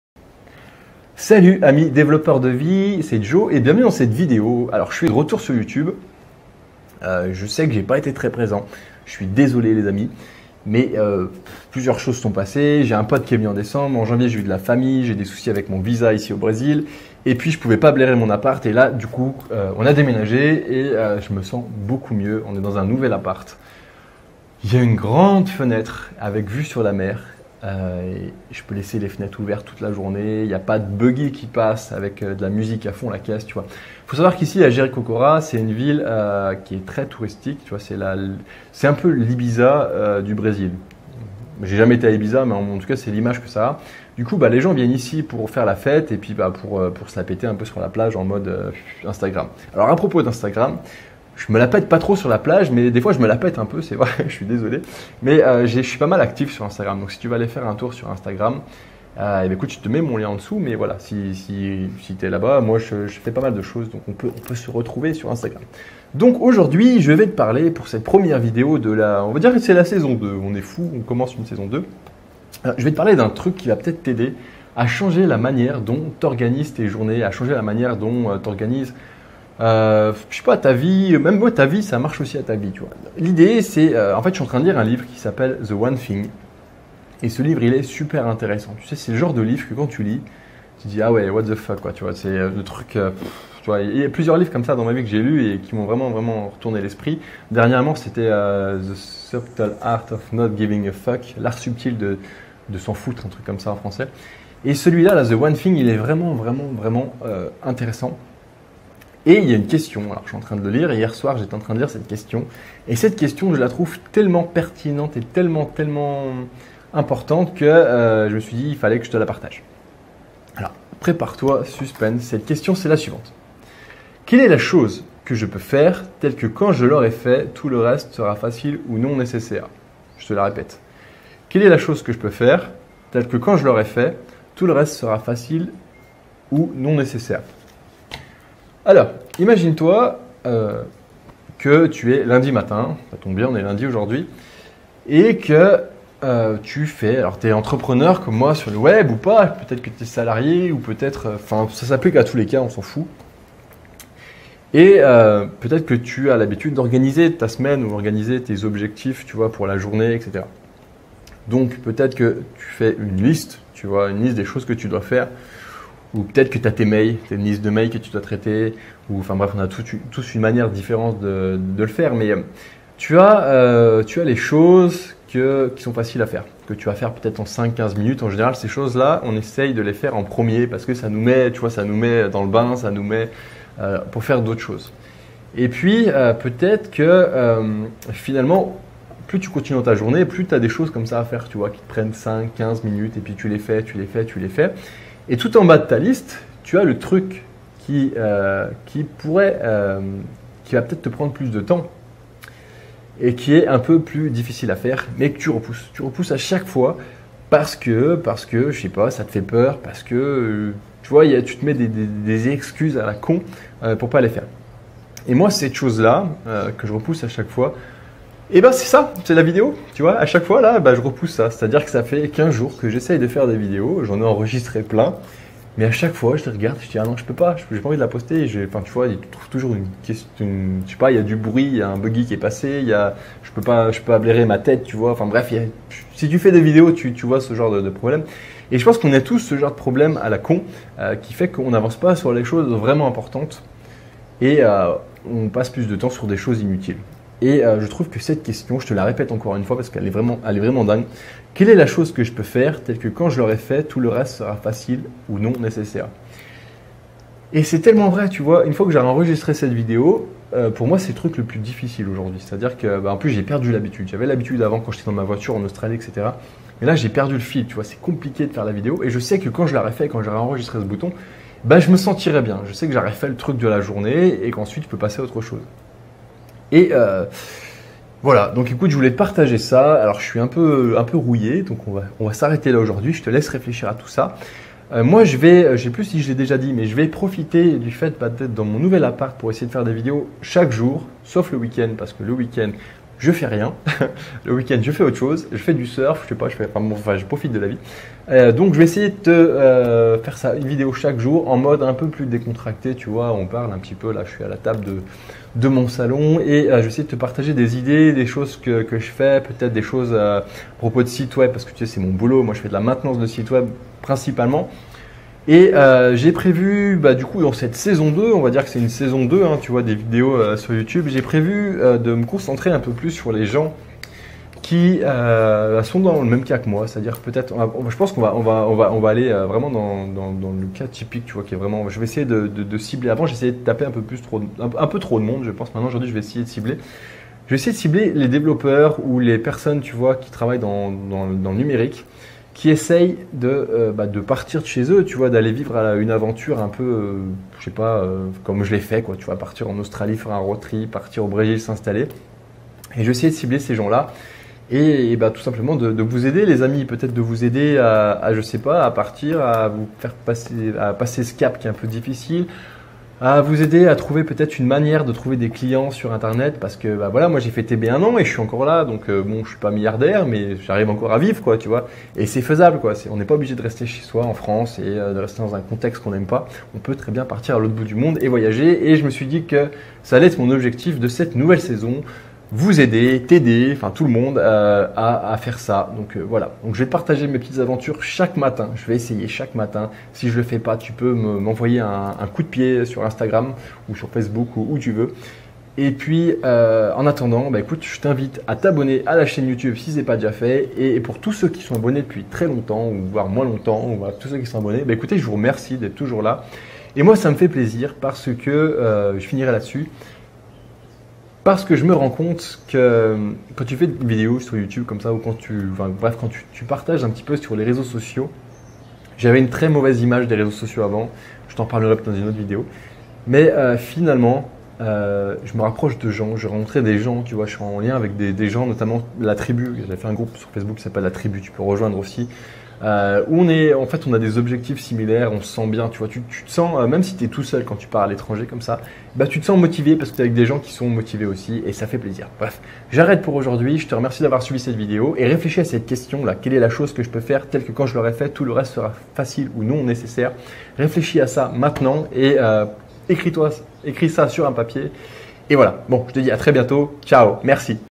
La QUESTION qui change TA journée
👇 Curieux ? plus d'infos en dessous 👇

Je suis de retour sur Youtube !
Aujourd'hui je te parle d'une question incroyable qui j'ai trouvé dans le livre "The One thing" de Garry Keller.
Cette question est : "Quelle est la chose que je peux faire, tel que quand je l'aurai fait, tout le reste sera facile ou non nécessaire ?".
Pose toi cette question chaque matin, et je suis convaincu que tu avanceras 10 fois plus vite dans tes objectifs.

Lien vers le livre